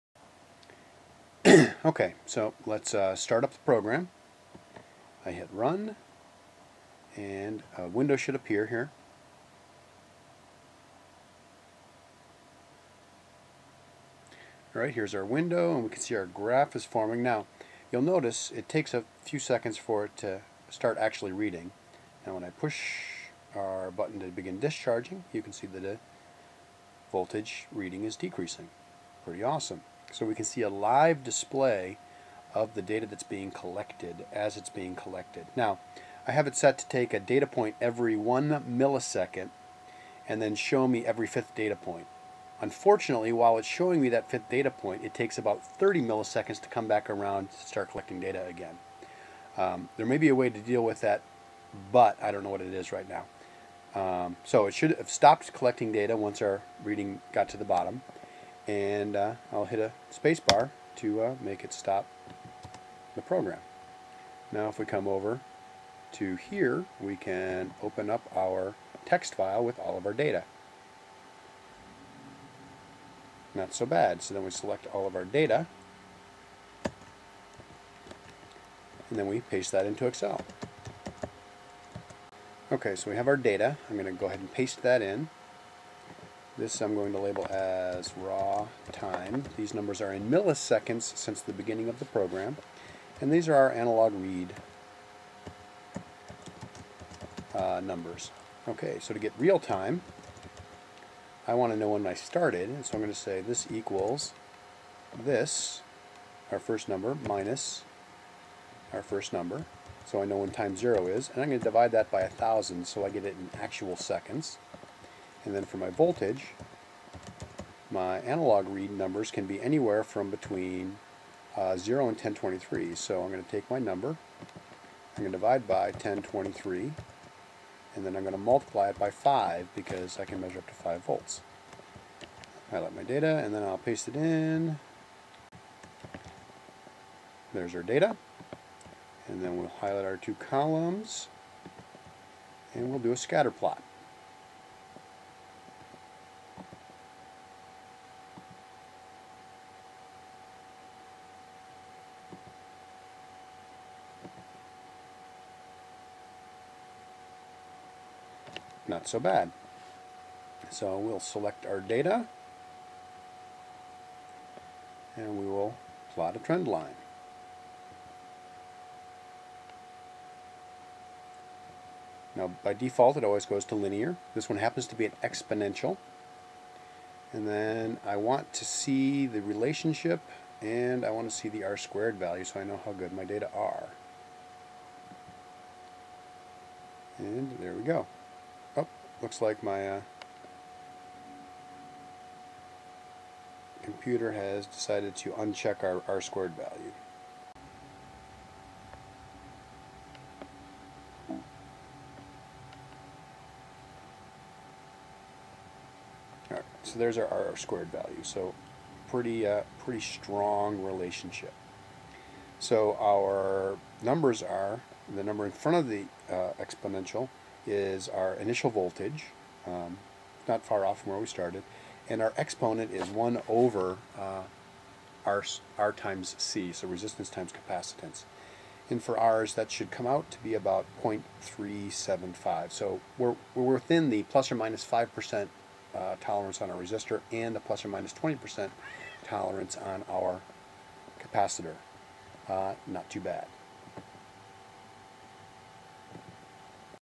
Arduino as Lab Sensor - Proof of Concept
Just a quick video created as a proof of concept for a project to create Open Source probeware for science classrooms.